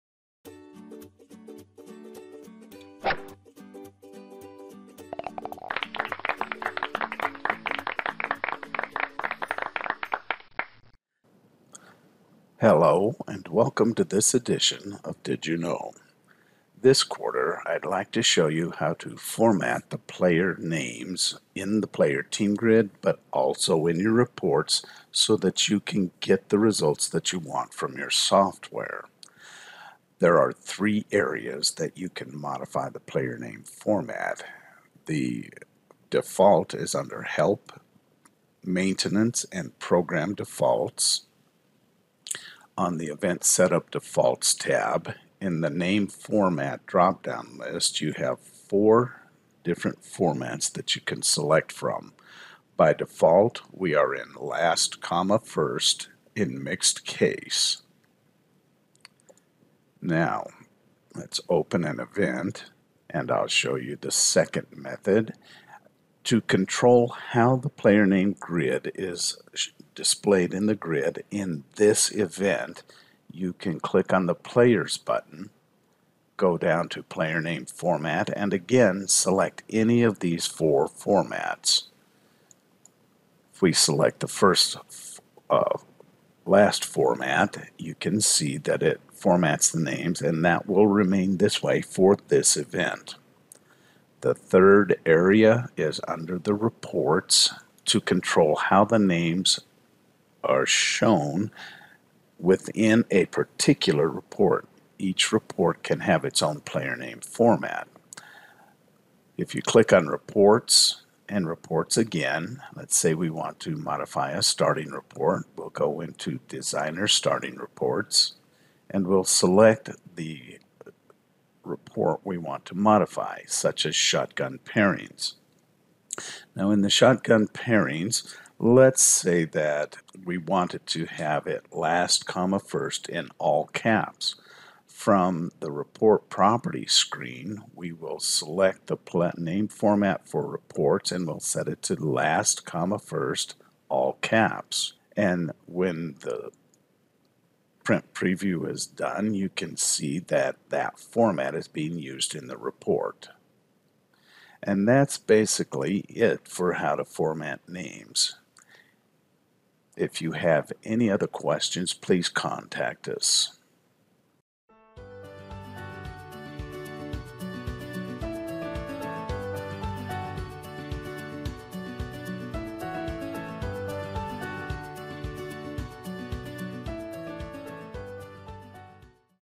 Player Name Formatting
This video will show you how to format your player names in the ViPer Web App Player Grid and Reports.